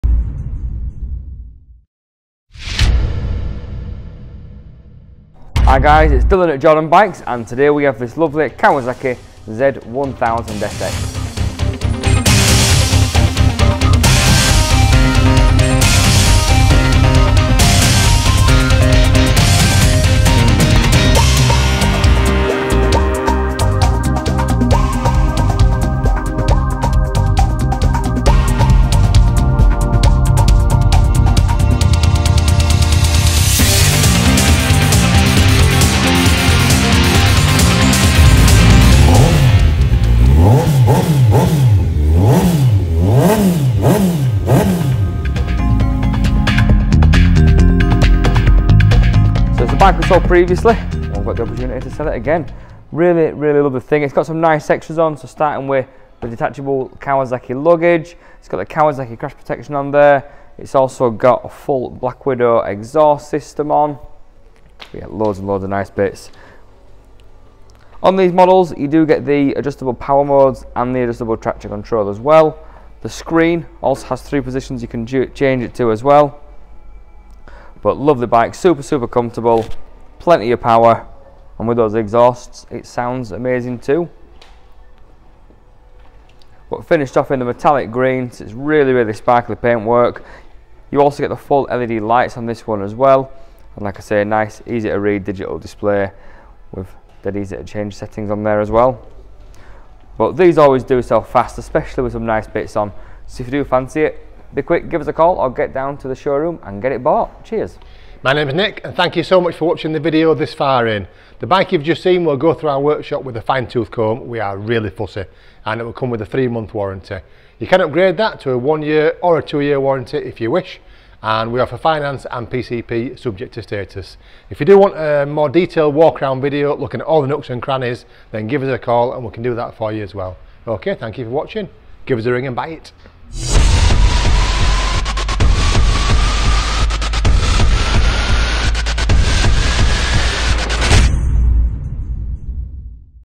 JORDAN BIKES- For Sale, Kawasaki Z1000SX ABS, 2019 19 plate, 11,783 miles, £8490
For Sale, Kawasaki Z1000SX ABS, 2019 '19' plate with only 11,783 miles on the clock. This is an absolutely stunning bike in a great colour. The Z1000SX has become one of the best sports tourers on the market offering a claimed 140bhp, with great handling and easily adjustable suspension for when you wish to carry luggage or a pillion. This updated version has full LED headlights, power modes, traction control, ABS, and an updated digital display. This bike also comes with the Kawasaki colour coded panniers, tail tidy, USB port, frame sliders, and a full Black Widow exhaust system that sounds fantastic. It will leave us after a fresh pair of tyres, so it's ready to be enjoyed. What a fantastic bike this one is. Yours for only £8490